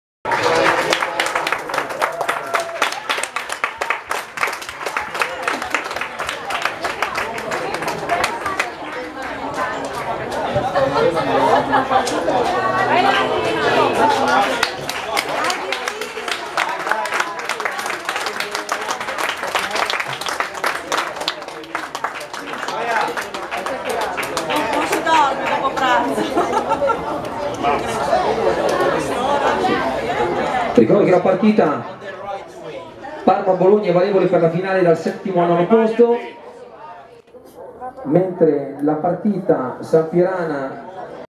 Pulcini 2008, 3° Memorial Betti, Parma-Bologna, ingresso squadre in campo
Campo Comunale Paolo Campodoni, San Piero in Bagno (FC), domenica 27 Maggio 2018